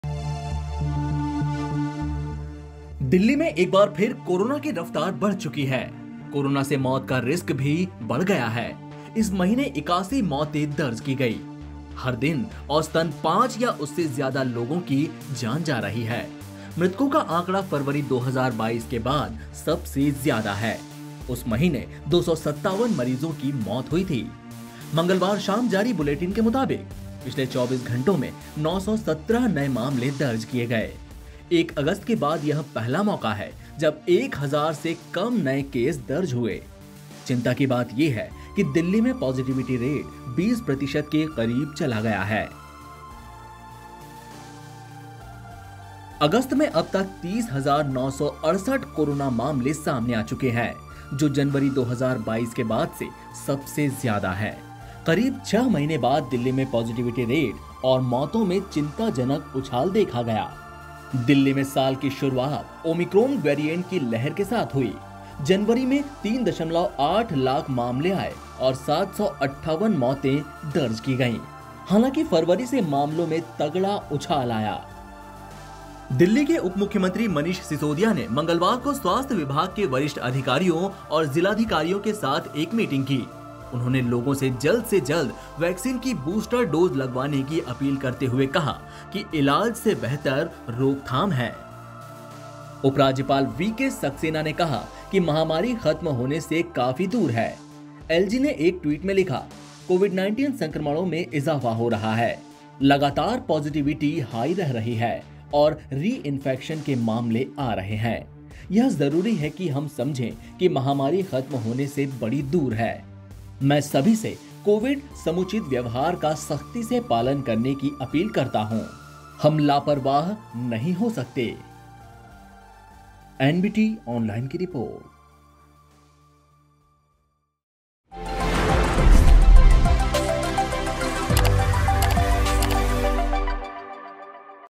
Coronavirus Update : Delhi में फिर बढ़े Corona Cases, Active Cases में उछाल | Corona Cases In Delhi
दिल्ली में एक बार फिर कोरोना की रफ्तार बढ़ रही है। कोरोना से मौत का रिस्‍क भी बढ़ा गया है। इस महीने 81 मौतें दर्ज की गईं। हर दिन औसतन पांच या उससे ज्‍यादा लोगों की जान गई। मृतकों का आंकड़ा फरवरी 2022 के बाद सबसे ज्‍यादा है। उस महीने 257 मरीजों की मौत हुई थी। मंगलवार शाम जारी बुलेटिन के मुताबिक, पिछले 24 घंटों में 917 नए मामले सामने आए। 1 अगस्‍त के बाद यह पहला मौका है जब 1,000 से कम नए केस दर्ज हुए। चिंता की बात ये है कि दिल्ली में पॉजिटिविटी रेट 20 प्रतिशत के करीब चला गया है।